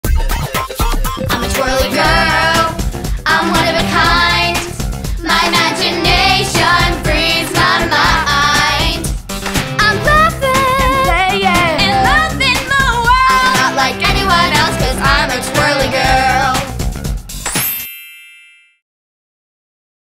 You Won't Believe How Our Clothing Comes Gift Wrapped
TwirlyGirl's girls clothing are unique and creative.  So it should come as no surprise that our wrapping is just as awesome.